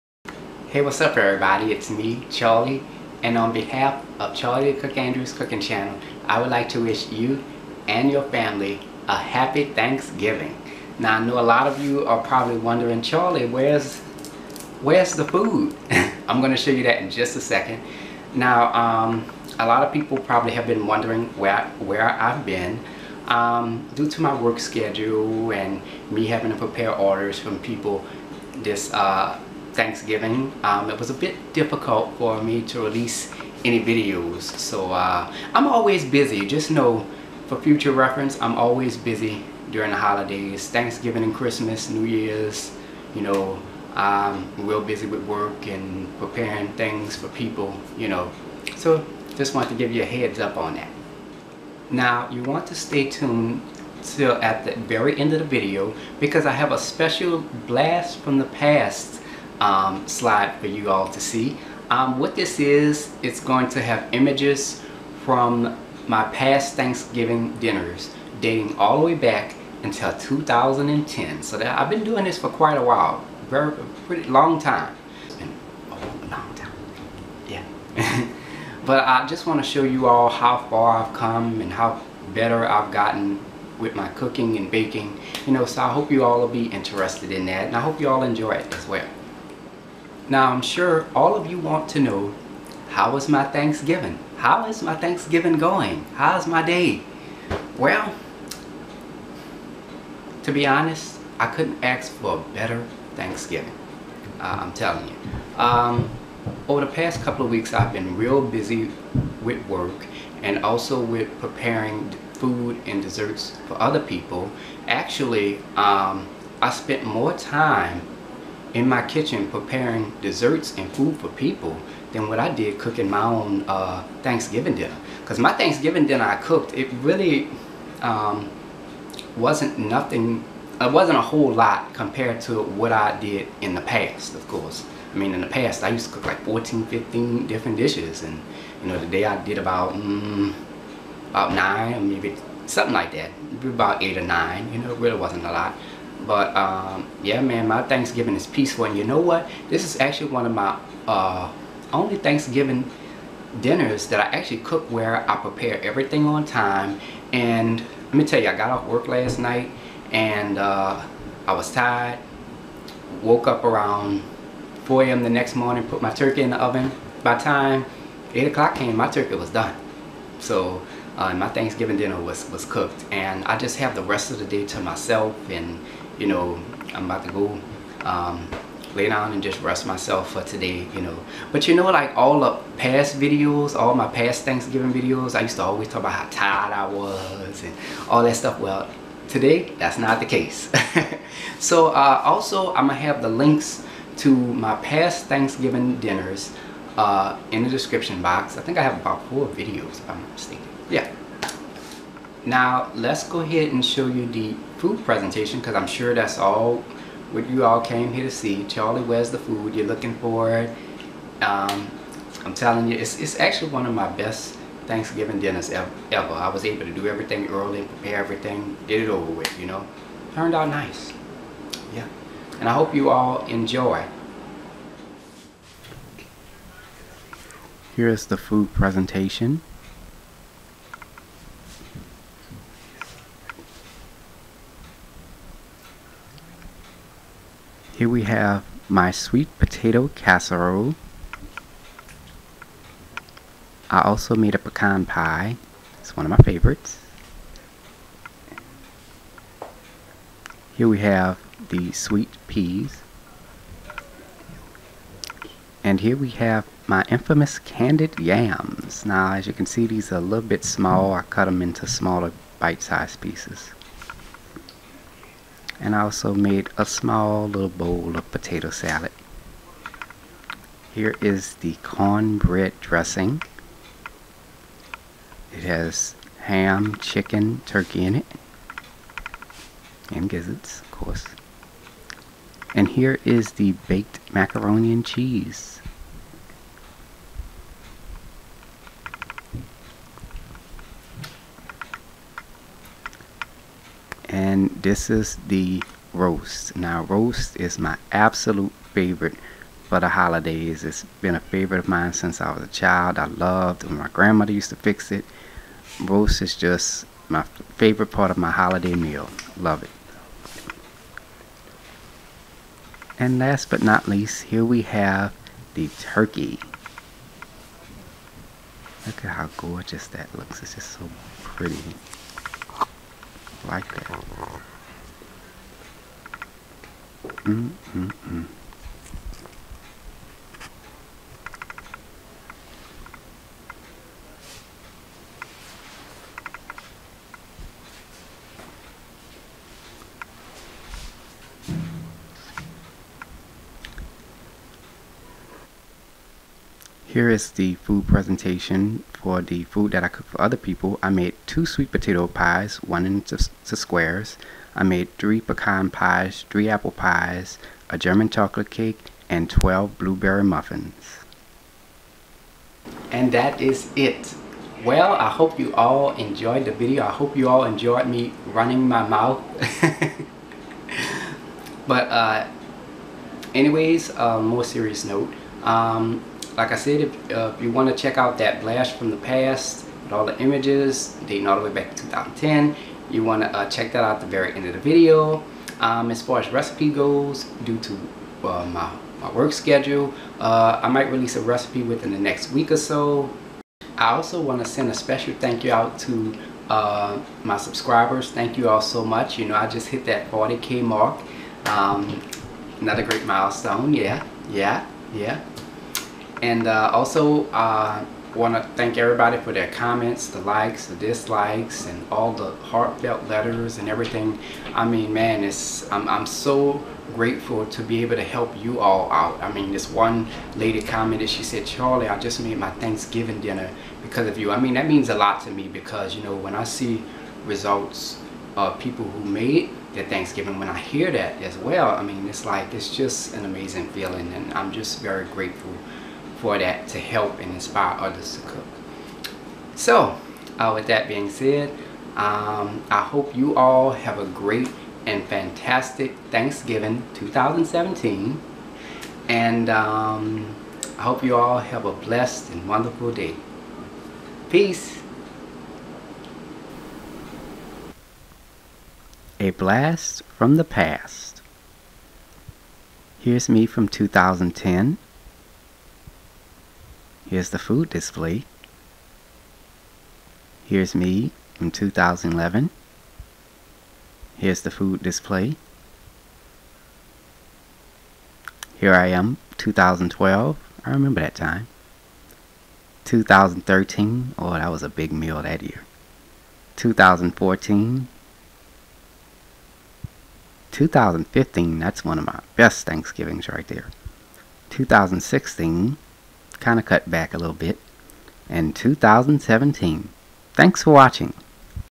Charlie's Thanksgiving 2017 Video
Charlie's Thanksgiving Dinner 2017.

A Blast from the Past! Thanksgiving dinner videos from 2014, 2015 and 2016. Enjoy!

$CharlieA504